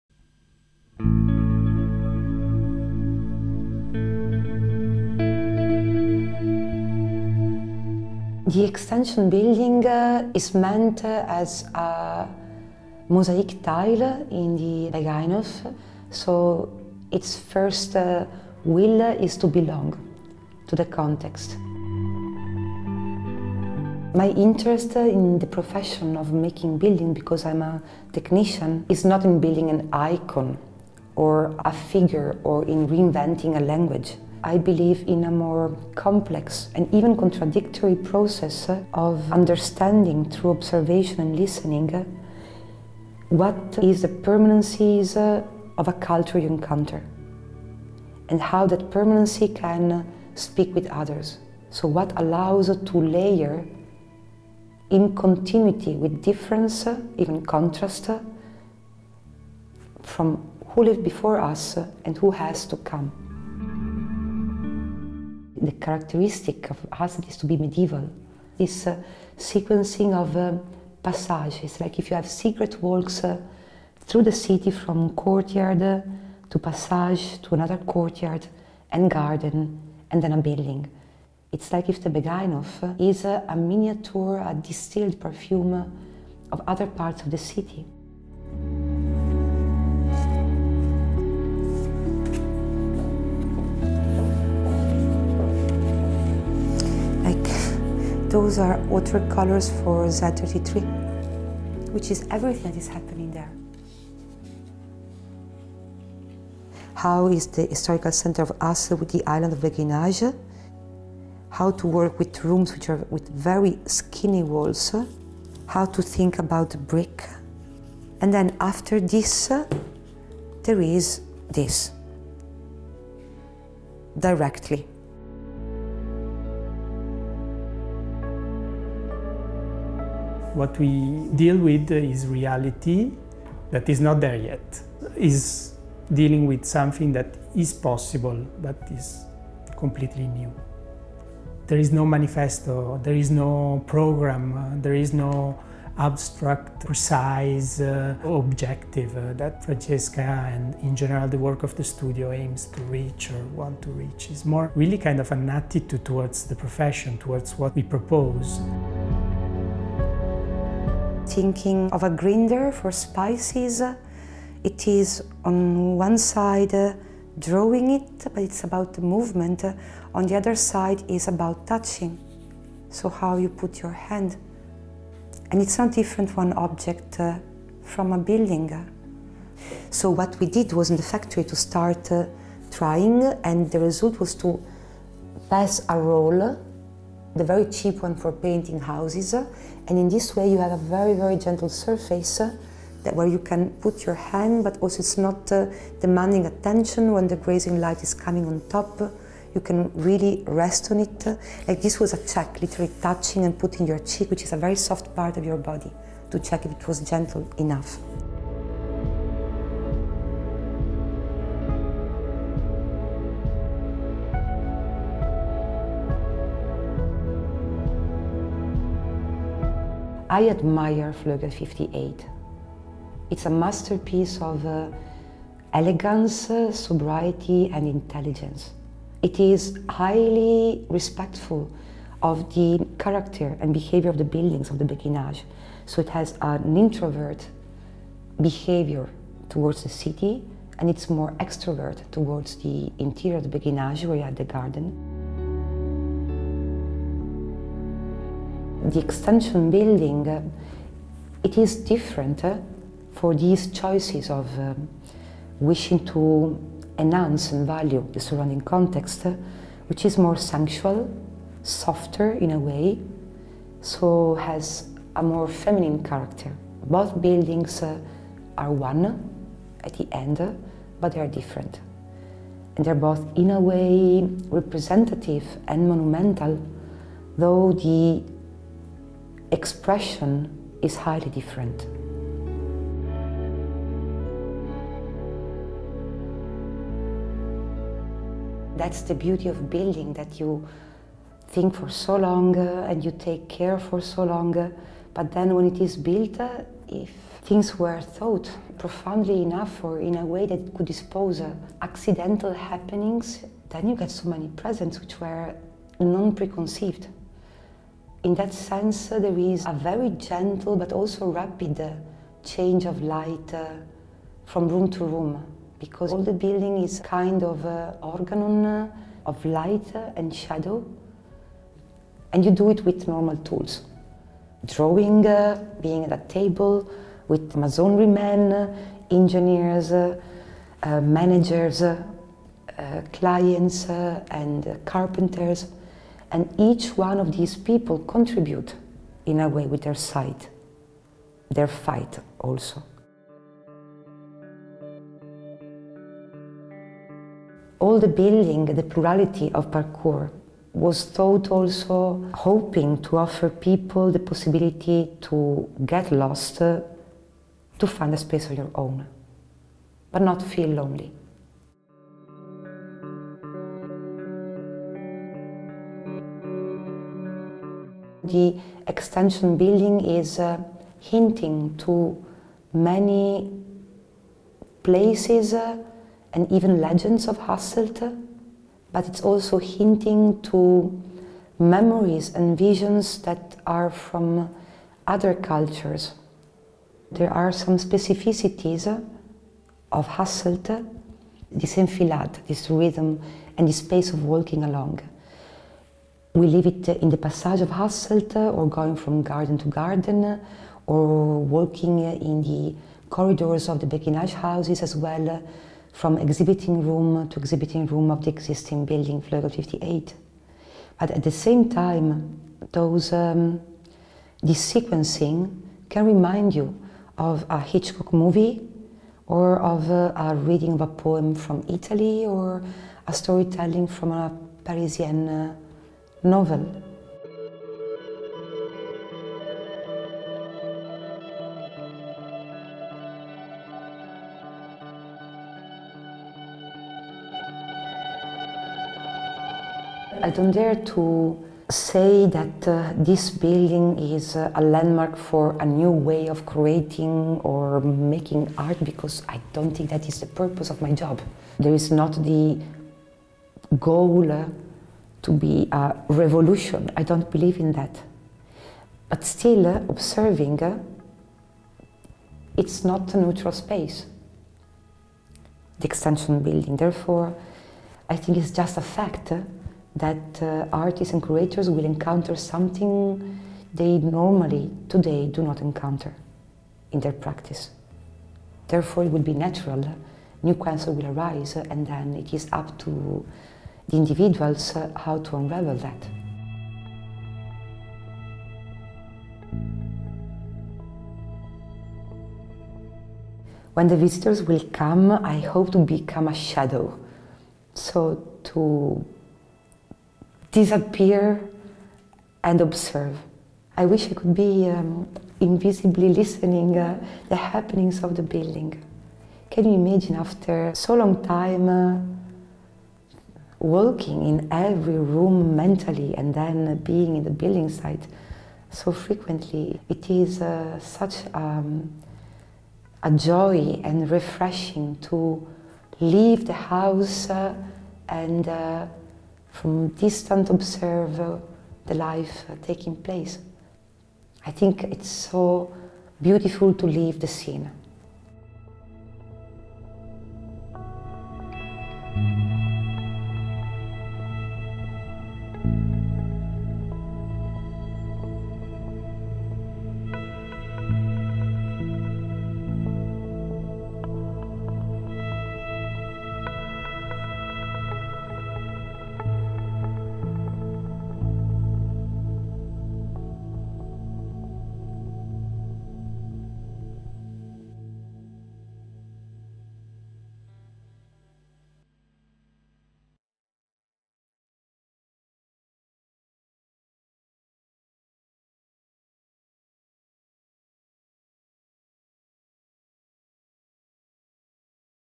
Wing 19 - A new building by Francesca Torzo for Z33 (Hasselt, BE)
Vleugel 19 (Wing 19) is the new exhibition building for Z33 - House for Contemporary Art, Design and Architecture. This is where Italian architect Francesca Torzo puts forward fundamental ideas about the nature and positioning itself. In addition, the building responds to the existing context, with Vleugel 58 (Wing 58), the Jenever museum and the adjoining beguinage. The result is a building that challenges visitors, curators and artists. The audience is not presented with virgin white boxes, but spaces with an outspoken character, which turns the building into a gift, not only for the art that will be displayed there, but also for the visitors and the city.

Architect Francesca Torzo for Z33, House for Contemporary Art, Design and Architecture in Hasselt (BE)

Videography by Studio Liese Lattrez
Graphic Design by Studio De Ronners
Special thanks to Francesca Torzo and Marco Guerra
Interview by Veerle Ausloos